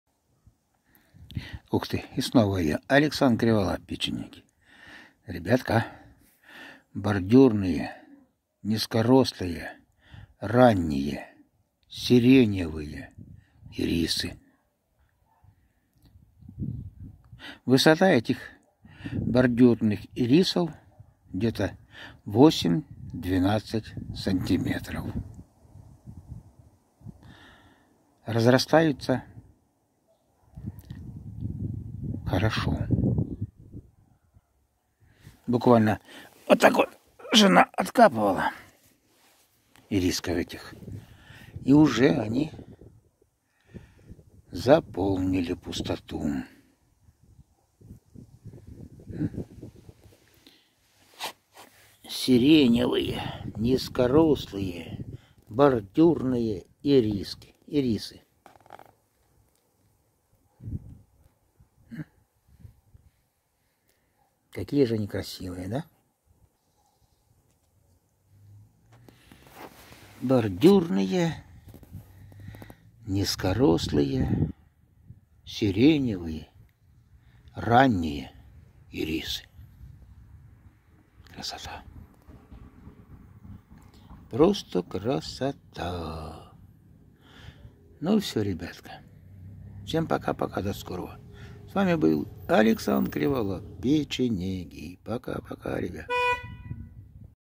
ЦВЕТЫ БОРДЮРНЫЕ НИЗКОРОСЛЫЕ РАННИЕ СИРЕНЕВЫЕ ИРИСЫ 2021 ГОД #100
україна АлександрКриволап цветыбордюрныенизкорослыесиреневыеирисы сад БОРДЮРНЫЕ,НИЗКОРОСЛЫЕ,РАННИЕ,СИРЕНЕВЫЕ ИРИСЫ 2021 ГОД .
 Видео БОРДЮРНЫЕ,НИЗКОРОСЛЫЕ,РАННИЕ,СИРЕНЕВЫЕ ИРИСЫ снимал 4 мая 2021 года .Цветы бордюрные ирисы это многолетние травянистые растения, Бордюрные Ирисы это короли сада .Высота этих бордюрных,ранних,низкорослых ирисов всего 8-10 сантиметров. Разводятся эти ирисы делением куста.